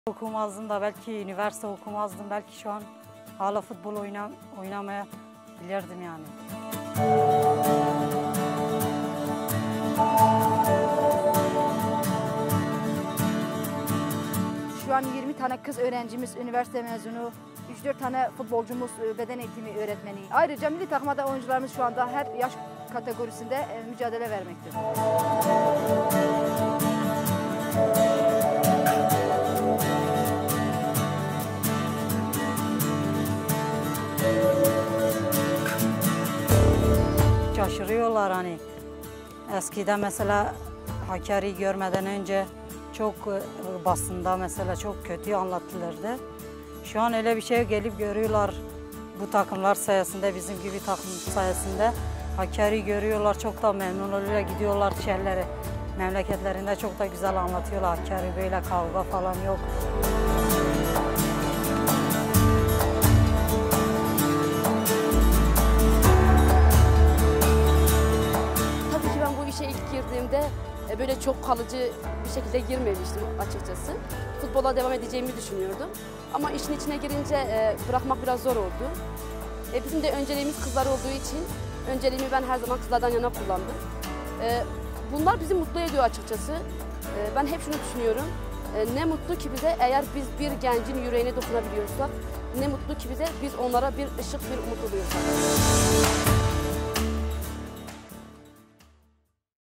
The story behind Turkish women's football team, Hakkarigucu Spor
Hakkarigucu Spor, a team in the Turkish women’s football league are challenging the taboos of playing football in Hakkari.